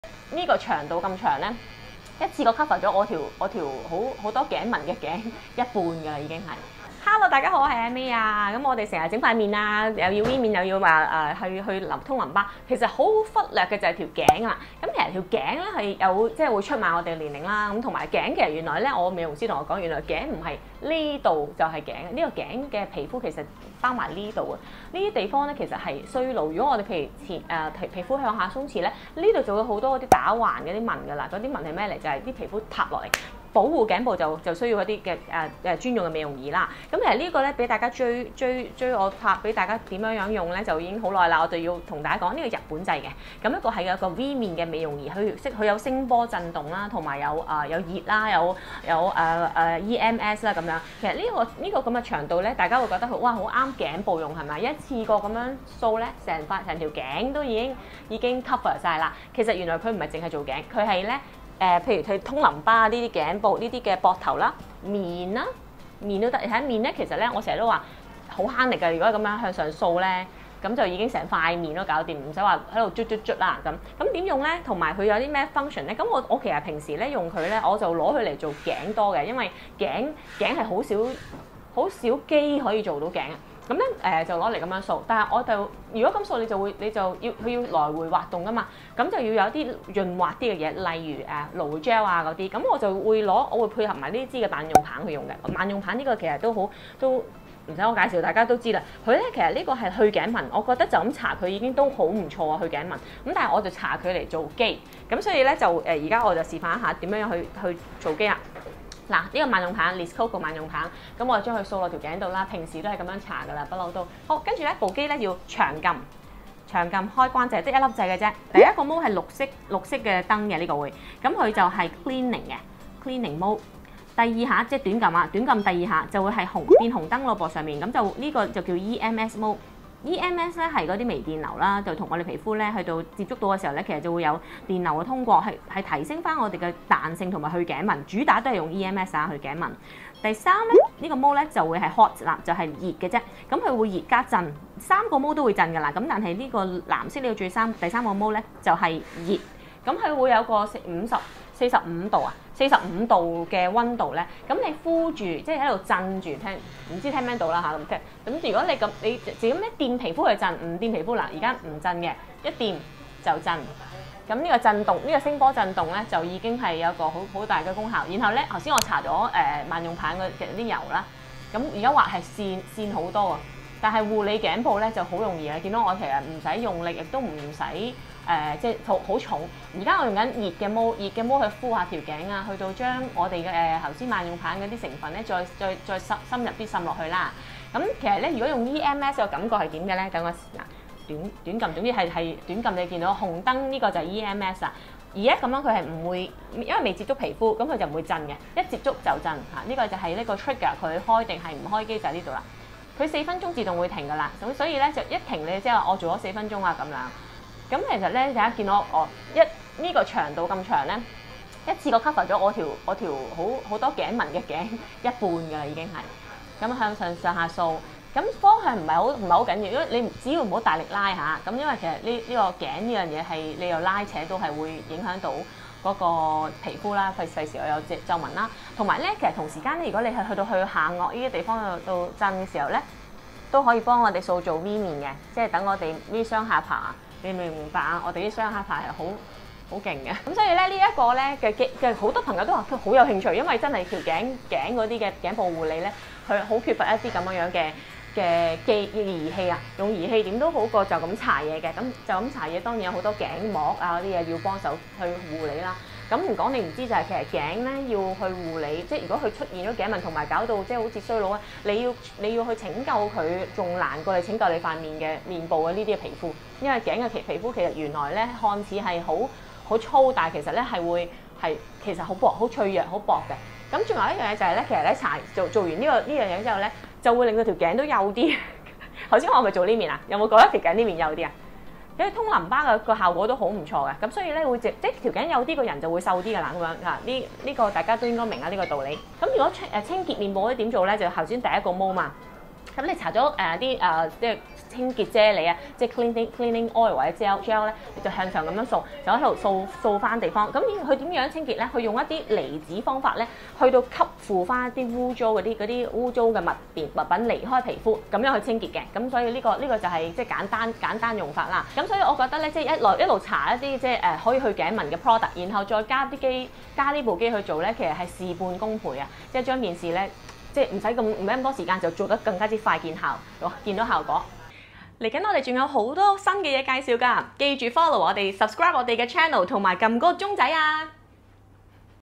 唔怕俾條頸出賣‼️日本 Carobasi 全效緊緻提拉美容儀
面部美容保養大家都做過，但你有冇試過同朋友相聚，你發得佢嘅面部肌膚非常紅潤後生有光澤，但就係條頸皺紋出賣咗佢…原因係大家好少照顧條頸‼️

去美容院嘅好處係全面性，美容師點樣都會推幾下下巴、照顧下頸紋…但係家中美容通常會忽視呢個重要環節，因為好少美容儀會做到頸，但呢個 Carobasi 嘅🇯🇵日本美容儀就做到呀😍

佢有聲波震動啦、熱感同EMS，個長度好啱頸部用，成條頸已經cover曬！佢唔係淨係做頸，例如去通淋巴、膊頭、面部都得。條勁幼啲，個人望落去都瘦啲㗎😂

3種模式分別為：
-潔淨亮膚
-EMS護膚
-恆溫保濕

8大核心功能，重現緊緻不留痕：
聲波震動
正負離子
深層清潔
雙輪按摩
刮痧排毒
三檔調節
EMS微電流
恆溫熱敷

重點功效：
彩光護膚
告別頸紋
重塑新肌
媲美技師頸部疏通按摩
均勻震動
刺激肌肉活力
疏通經絡
加速血液迴
收縮毛孔
改善膚色暗沉
提升肌膚彈性
淡化皺紋
深層觸及全面肌膚

日本製造